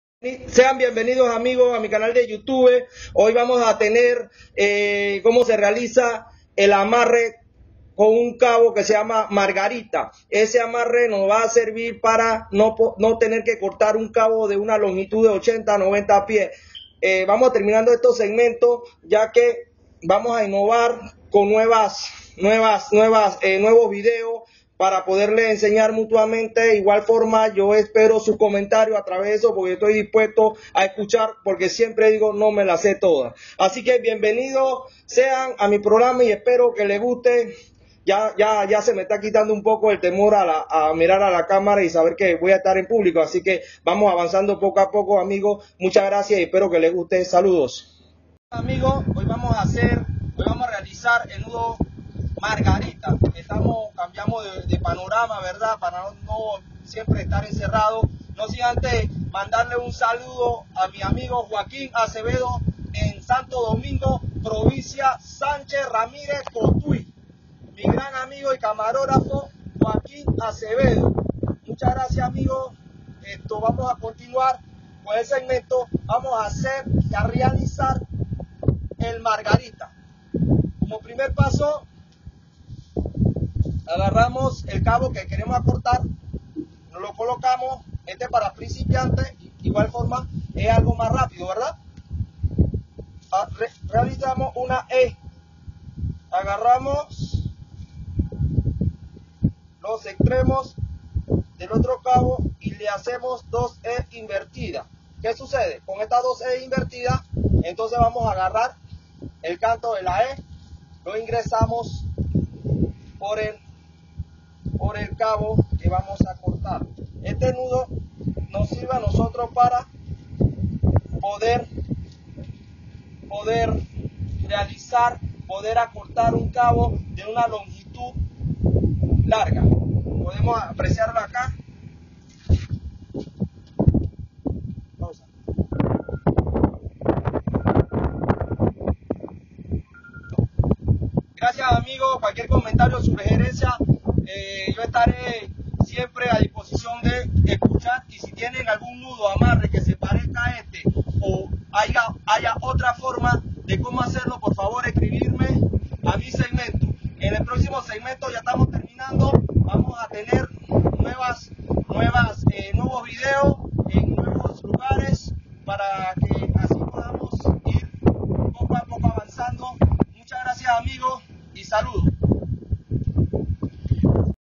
nudo Margarita %%
Acortando una soga sin usar un cuchillo..